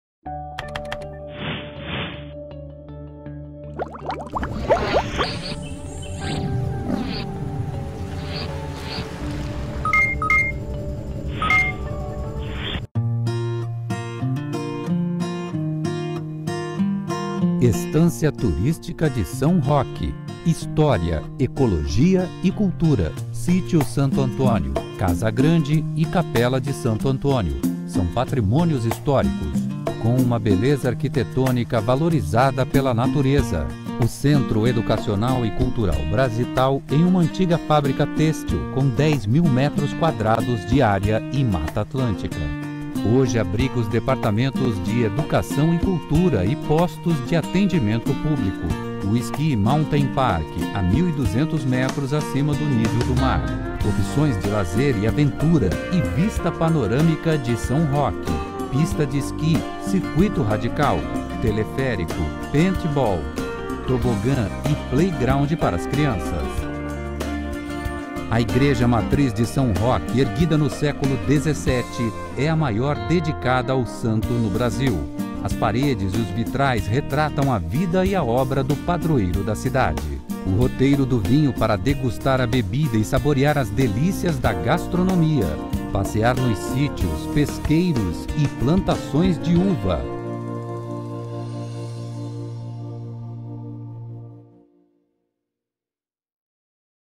Vídeo institucional da Aprecesp sobre a cidade de São Roque (2013)
Vídeo institucional da Associação das Prefeituras das Cidades Estância do Estado de São Paulo sobre a cidade de São Roque, 2013. Para mais informações sobre este vídeo, acesse

Acesse e veja todos os itens do Arquivo Histórico Digital de São Roque.